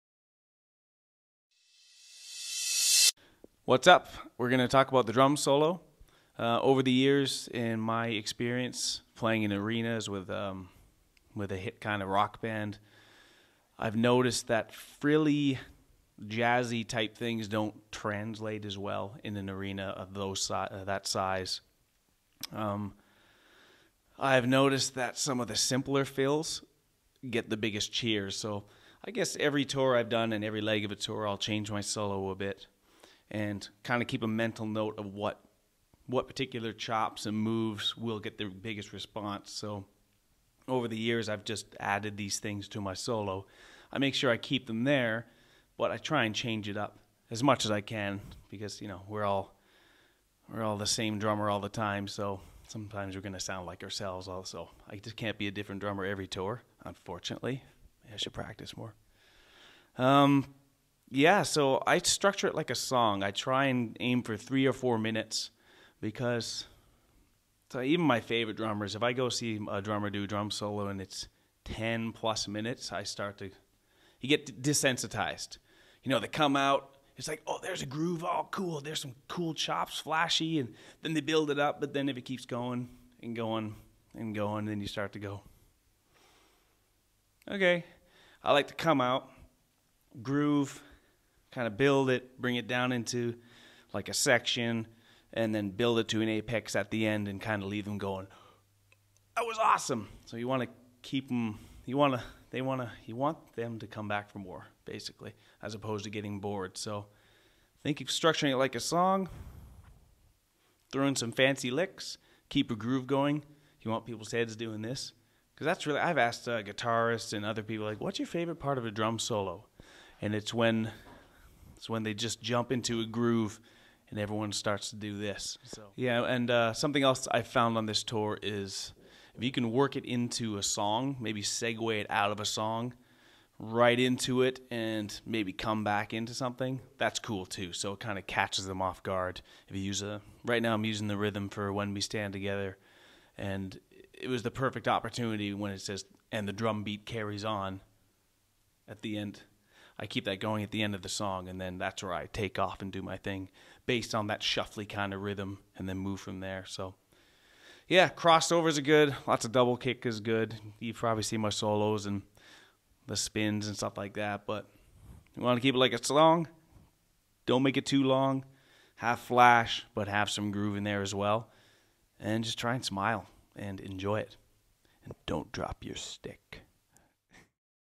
Nickelback's Daniel Adair on how to put together a drum solo
Nickelback drummer Daniel Adair tells Rhythm what he keeps in mind when putting together a drum solo.